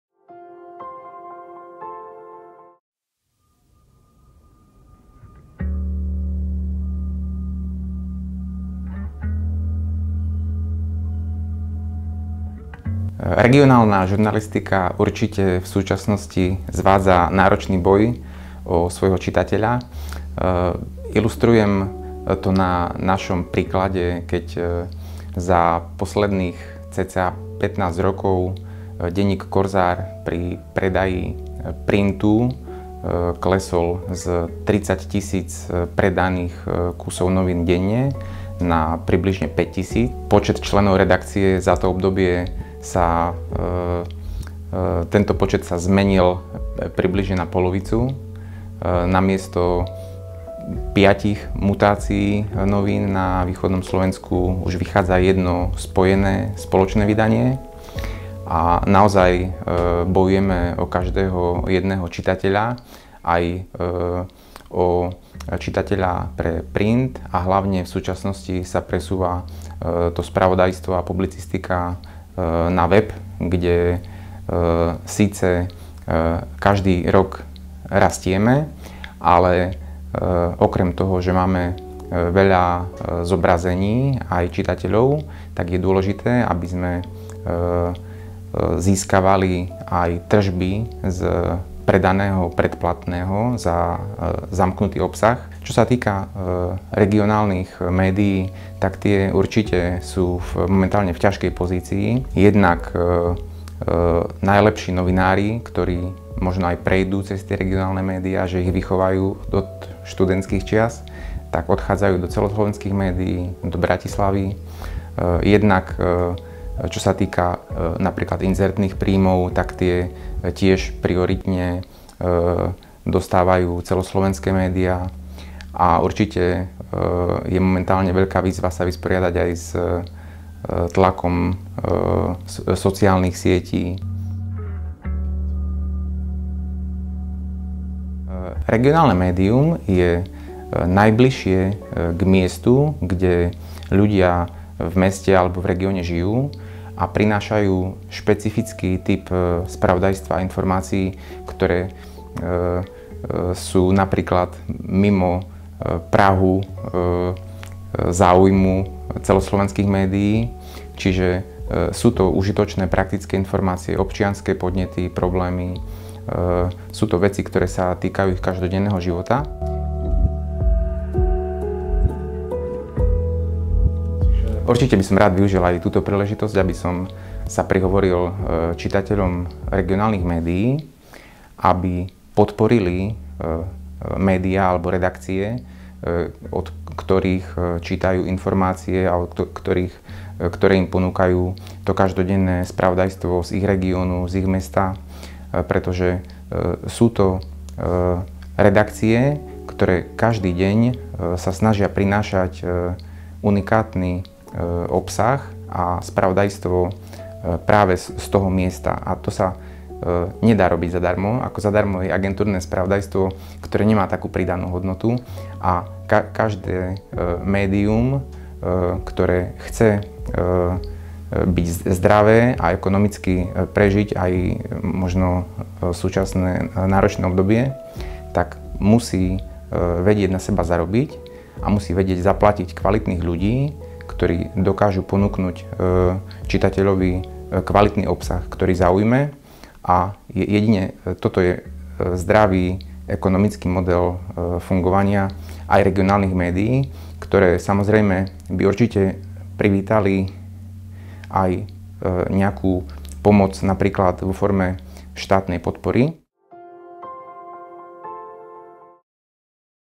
Lokálne noviny a aktuálne trendy/Jaroslav Vrábeľ
Jaroslav Vrábeľ, šéfredaktor regionálneho denníka Korzár, predstavuje súčasné trendy -a ich pozitívny aj negatívny vplyv na regionálne noviny.

Podľa jeho slov by občania mali podporovať lokálne noviny, pretože práve tie sa snažia prinášať unikátny obsah a spravodajstvo z daného miesta.